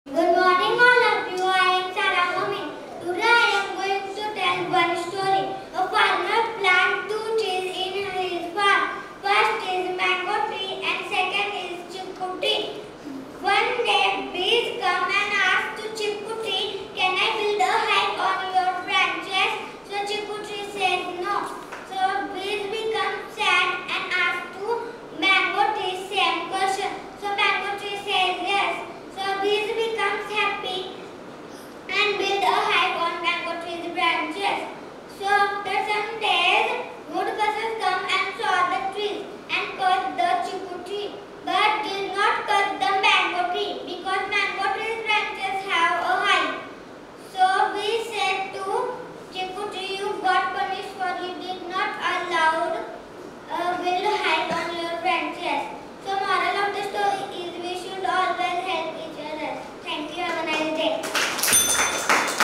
"The power of storytelling." Storytelling competition in Shri A. R. Tambe Sir's School
"Congratulations to all the students who participated in the storytelling competition! We are so proud of your confidence and stage courage. You all did a wonderful job of sharing your stories with us.

We know that storytelling can be a daunting task, but you all rose to the occasion and delivered amazing performances. You showed us that you are not afraid to speak up and share your ideas. This is a valuable skill that will serve you well in all areas of your life.

We hope that this competition has helped you to build your confidence and to become more comfortable speaking in front of others. We encourage you to continue to tell stories, whether it is in front of a crowd or just to your friends and family. Storytelling is a powerful way to connect with others and to share your unique voice.

Thank you again for participating in the storytelling competition. We are so proud of you all!"